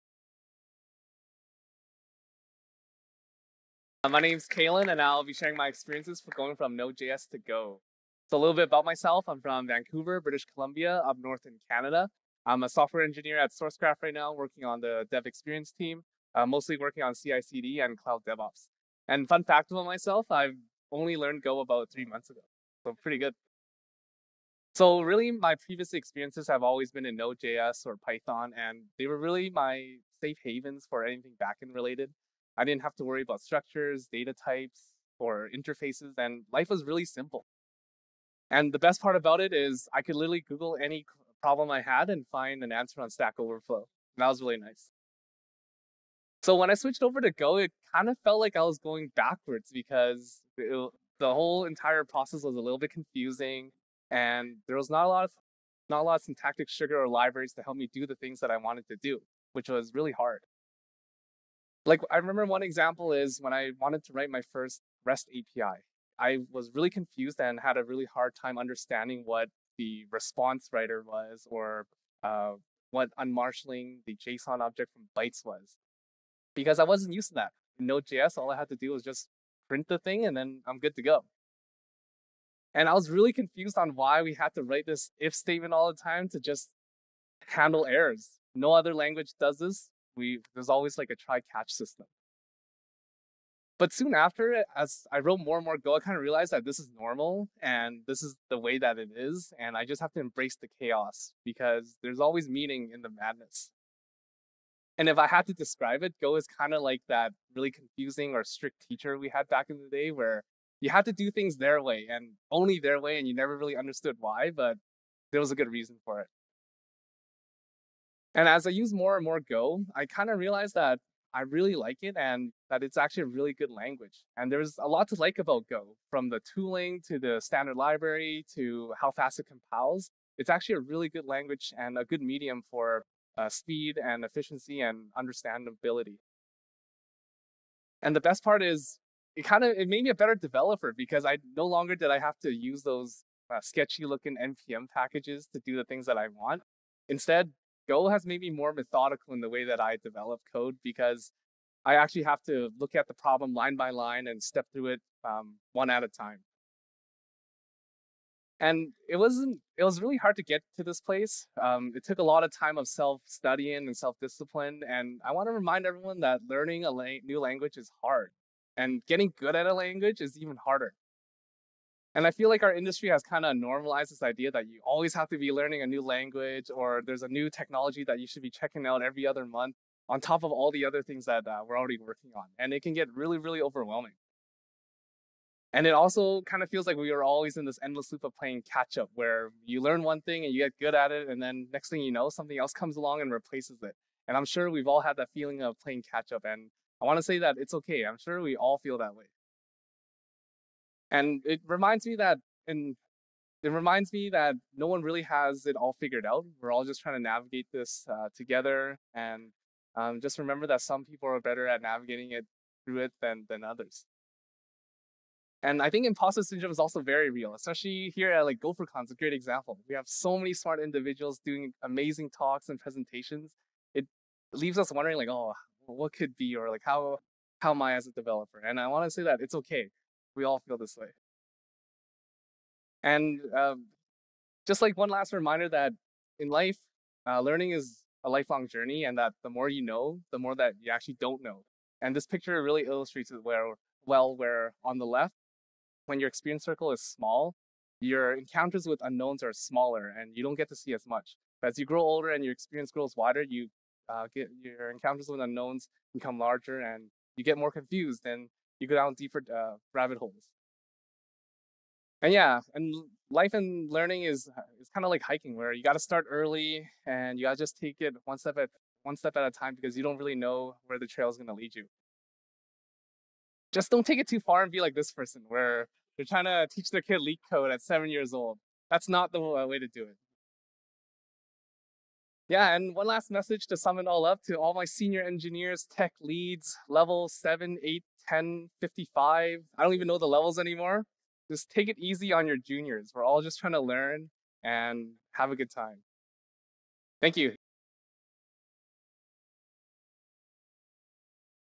GopherCon 2022: Making the Leap from NodeJS to Go - Kalan Chan
This Lightning Talk will be focused on my experiences going from NodeJS backends to Golang. I will be covering some of the weird pain points of Go and how different the language is compared to non-strongly typed languages. Afterward, I will explain why I think switching to Go was a good move, talk about all the positives, and tell how Go has made me a better developer.